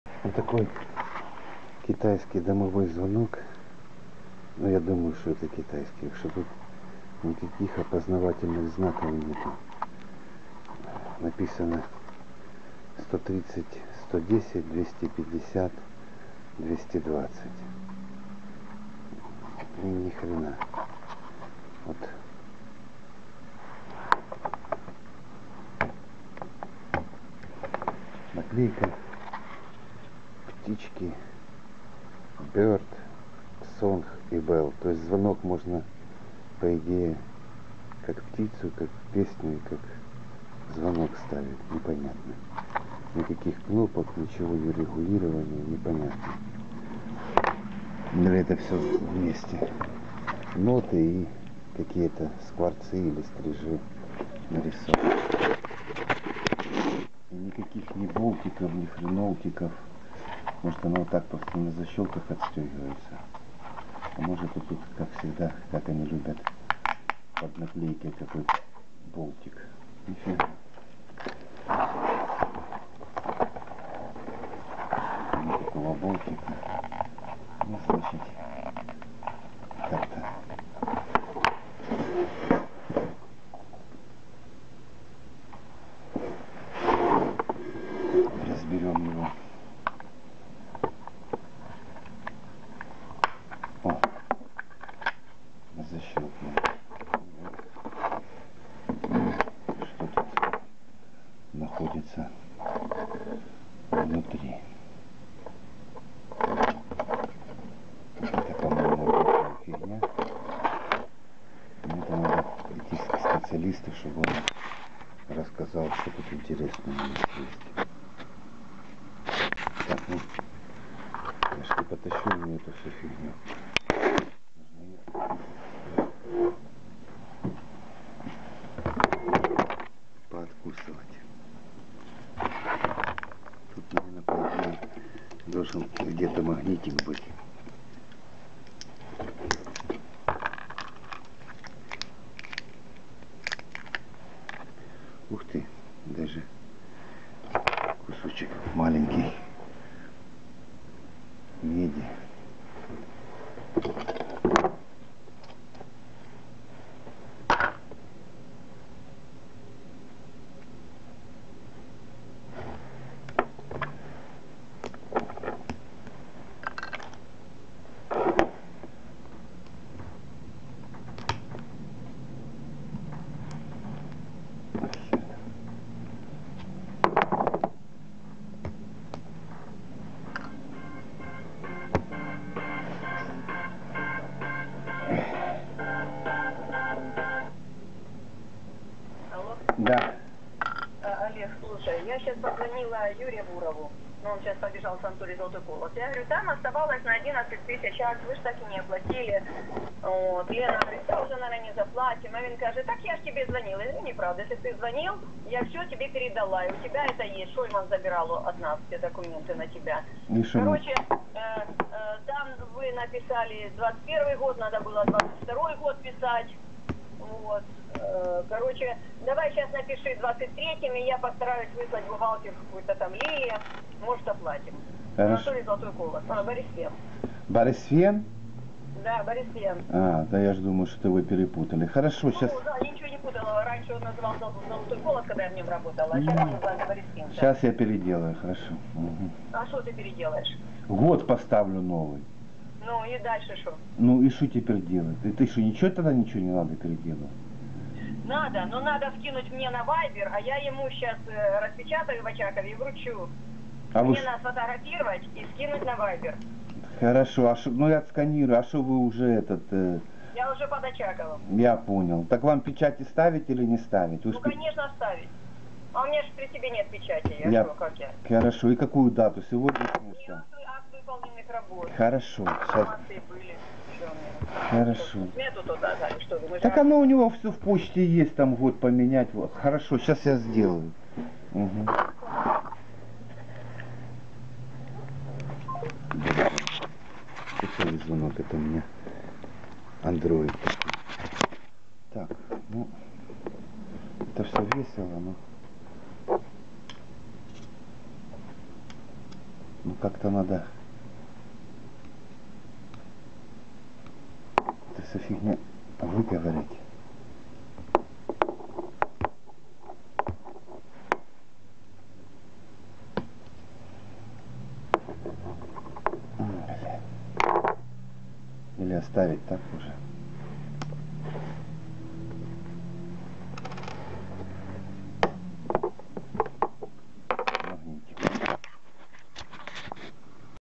Что там внутри. S02E07. Разборка Bird Song Bell
Как разломать китайский дверной звонок своими руками?
Жертва - Bird Song Bell 130-110/250-220.
Продолжение следует...